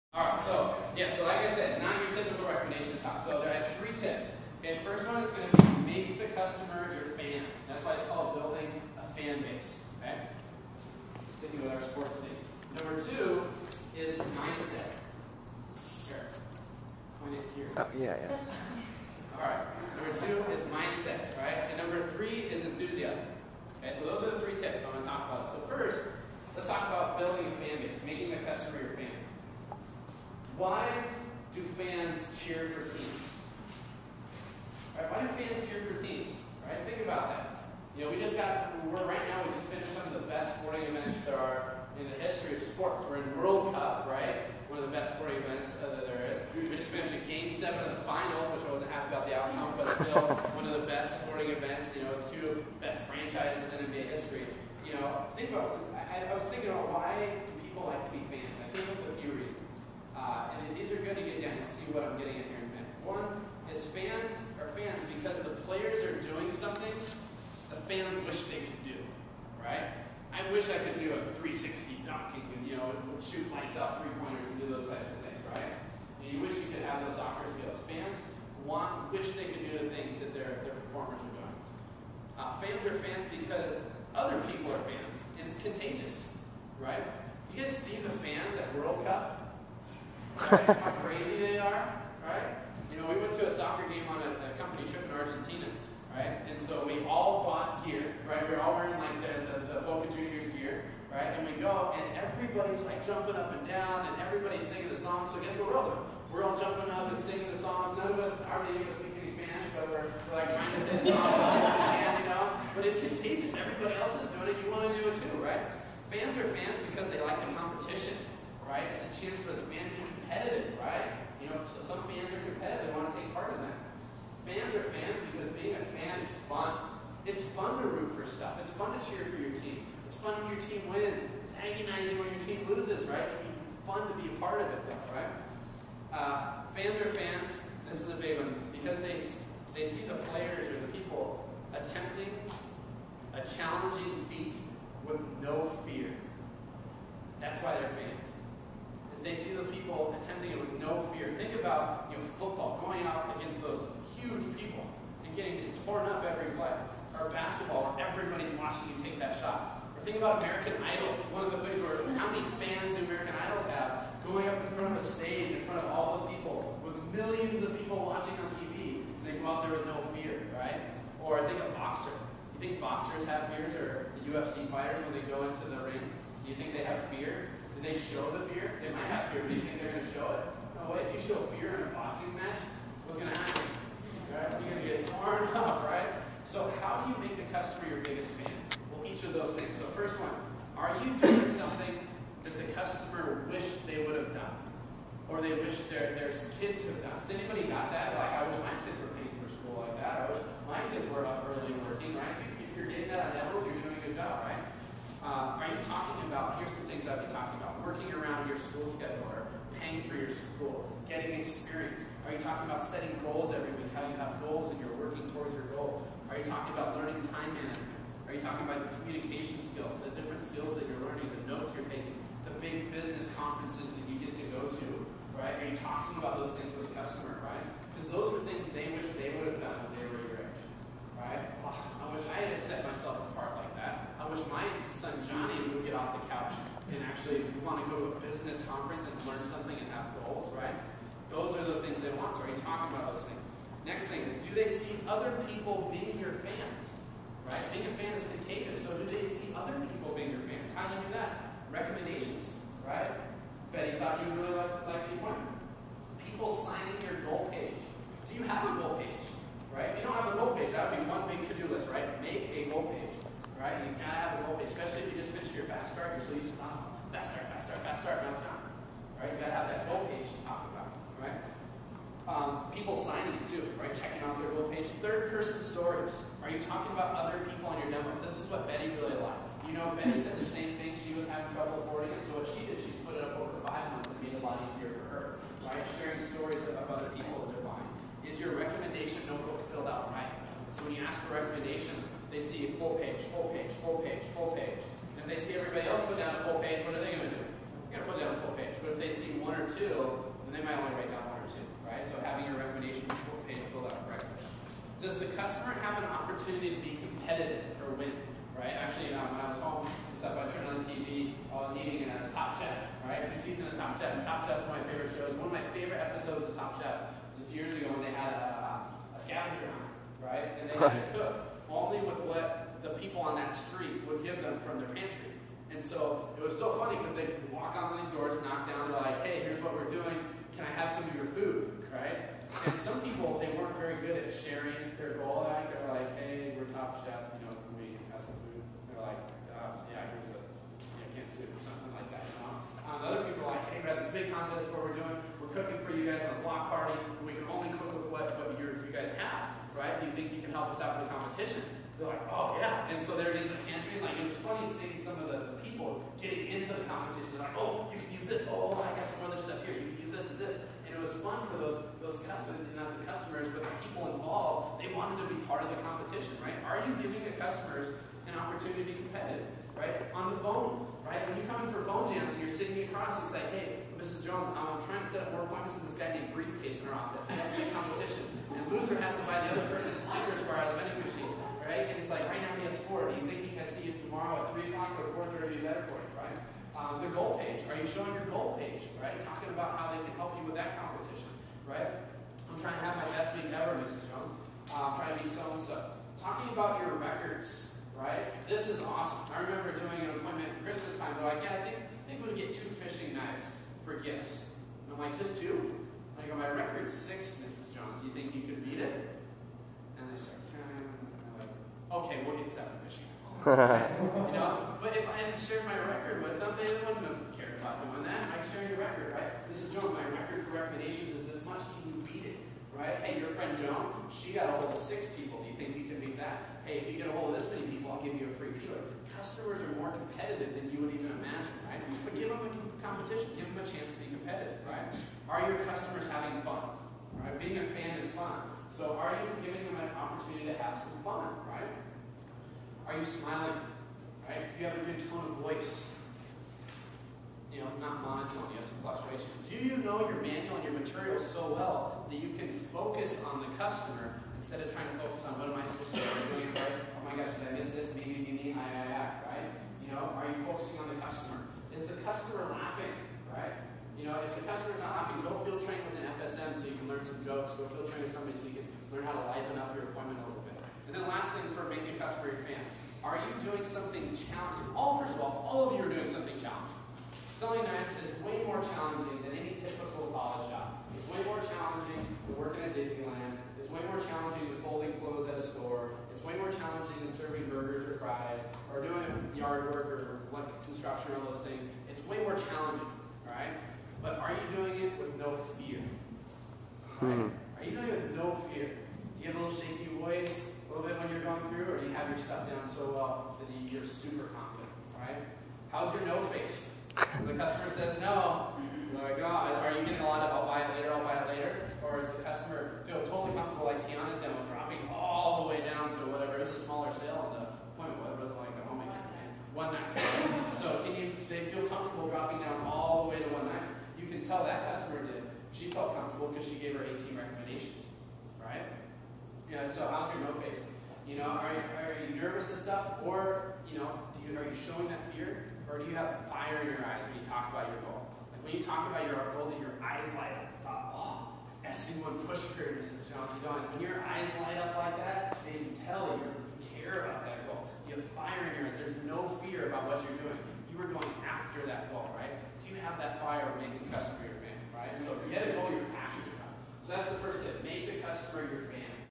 Abundant Leads Part 1
How to increase the amount of recommendations and yes' you get from each appointment.  From the "Game On" team meeting summer 2010.